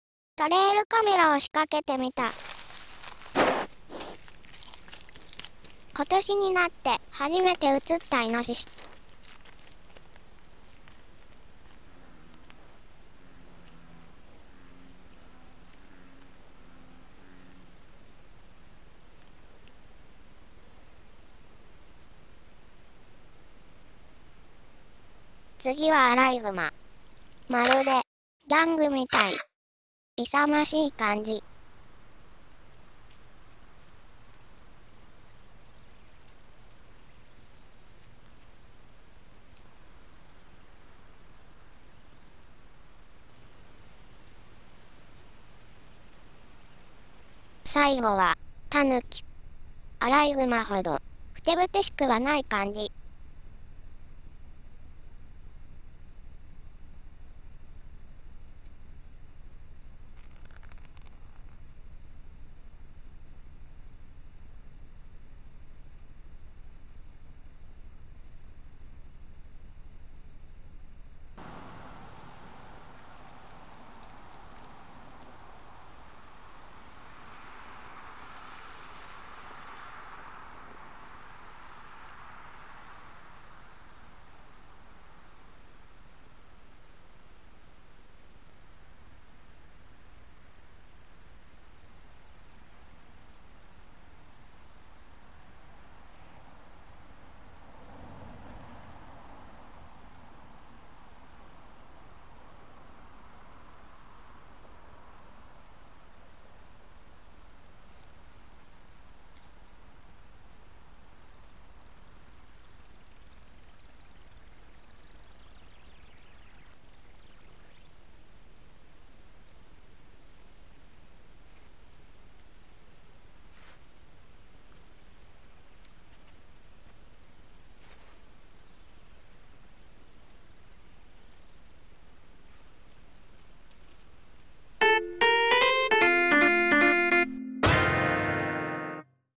今年もイノシシとアライグマとタヌキが来たよ！
トレイルカメラを設置したのは2020年の８月からです。アライグマもタヌキも昨年は１回だけしか映っていませんでした。しかしながら、そのタヌキと思っていた動物は、実はムジナだったことが今回わかりました(笑)
冬場にぱったり見かけなくなっていた動物たちも、３月中旬以降は頻繁にやって来るようになりました。この先もカメラの映像をチェックするのが楽しみです。

動画のオープニングのナレーションキャラクター
エリちゃんのLINEスタンプを作ってみました。